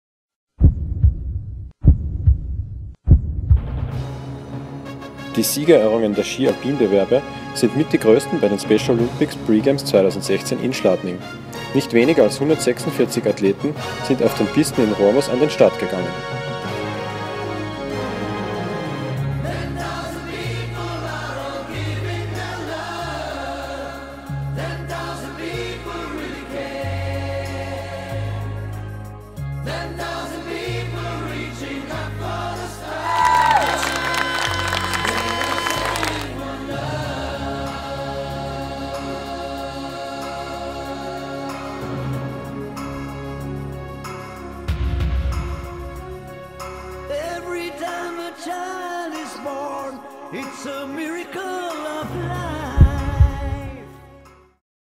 Siegerehrung SkiAlpin
Die Siegerehrungen der Ski Alpin-Bewerbe sind mit die größten bei den Special Olympics Pre-Games in Schladming 2016. Nicht weniger als 146 Athleten sind auf den Pisten in Rohrmoos an den Start gegangen.